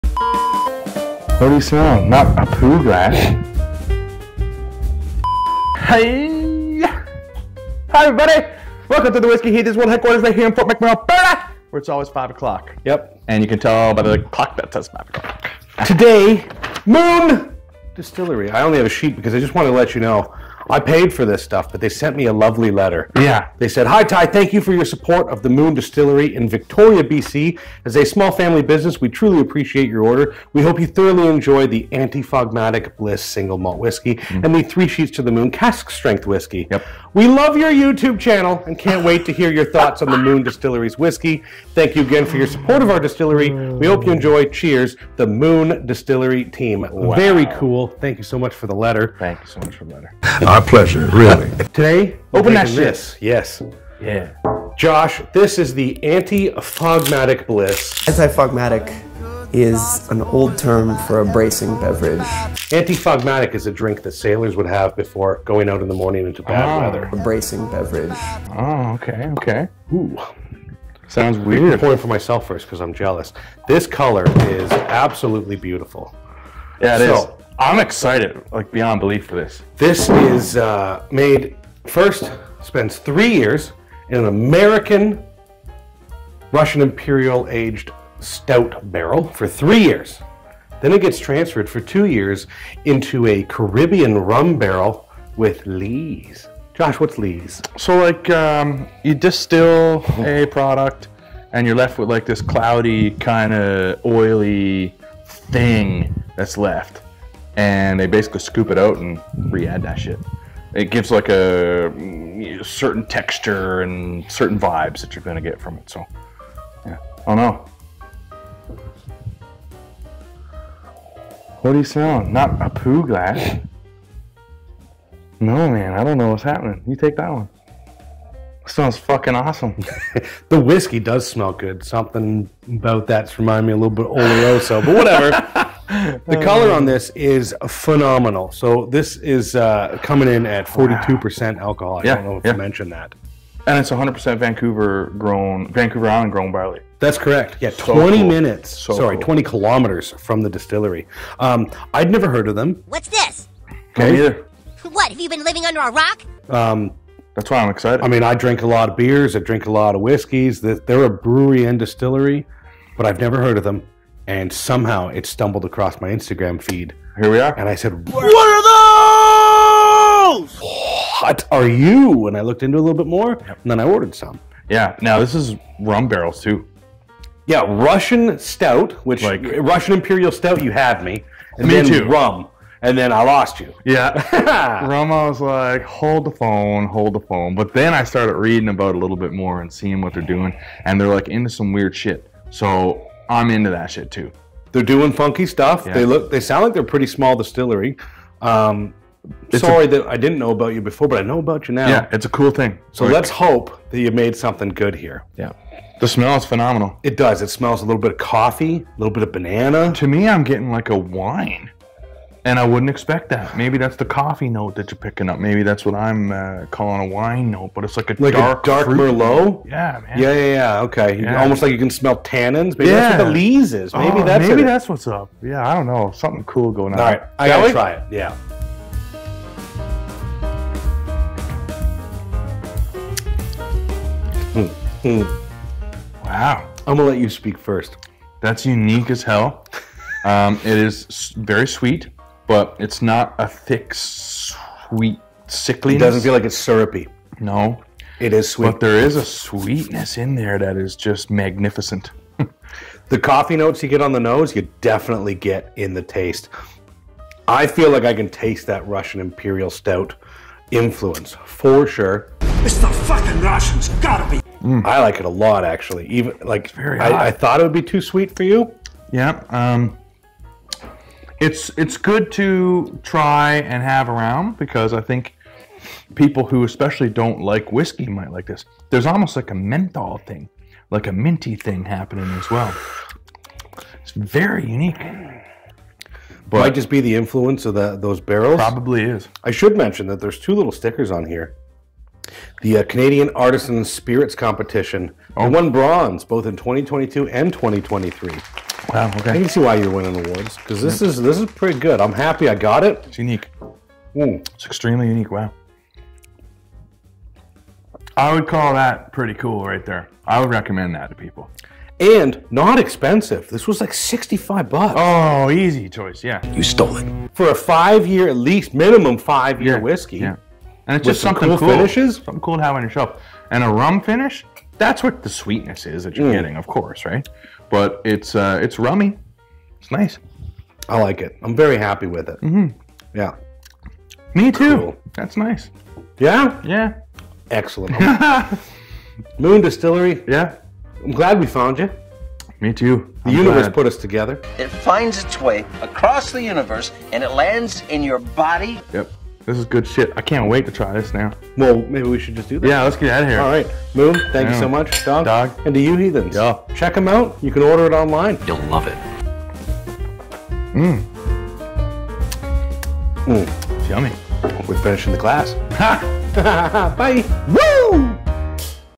Canadian Whisky Antifogmatic Bliss Finished in RUM Casks from the MOON
Single Malt Whisky inspired by Clay’s love of the lighter side of Speyside’s “lunchtime” Whiskys. Lightly filtered to round out this smooth sipping Whisky, made in our Victoria BC based Distillery.

Name: “Antifogmatic Bliss” Single Malt Whisky

Grain Bill: 100% Vancouver Island Grown Barley Malt

Still Type: Pot Still

Number of Times Distilled: Twice

Cask Type: Aged 3 years in American Oak, previously a Russian Imperial Stout aged barrel, then transferred into a Caribbean Rum barrel (with lees) for 2+ more years

Maturation Duration: First 5+ year old Whisky release!

ABV: 42%